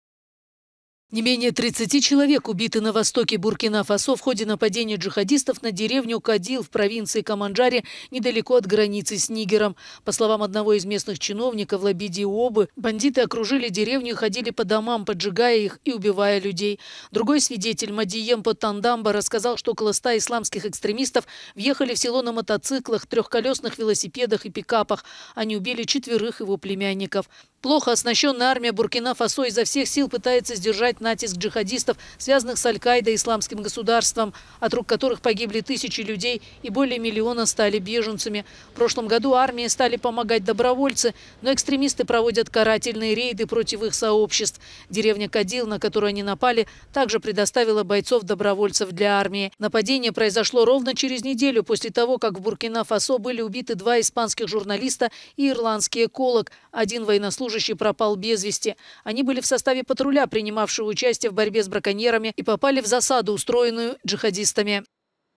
Буркина-Фасо: месть джихадистов
Джихадисты в Буркина-Фасо напали на деревню Кодил, сожгли дома и убили жителей. Их этой деревни ранее на помощь армии ушли добровольцы.